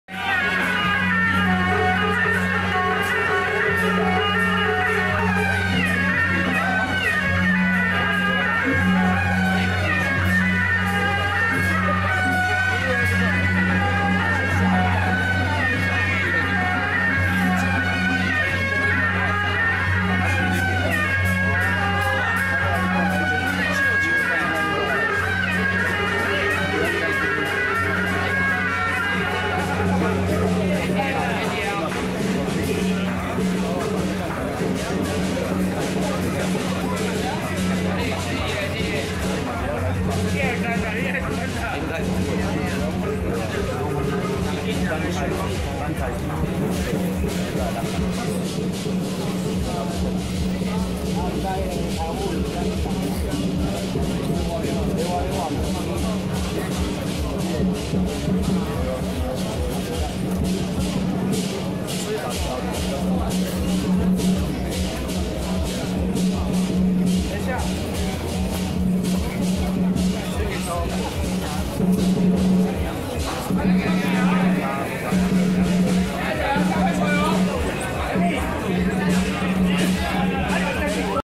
108三重天師府張府天師南巡進香回駕遶境【三重武聯堂-北極玄天上帝 聖駕(桃園劉家+新北文衡轎班會)】
三重武聯堂-北極玄天上帝 聖駕(桃園劉家+新北文衡轎班會)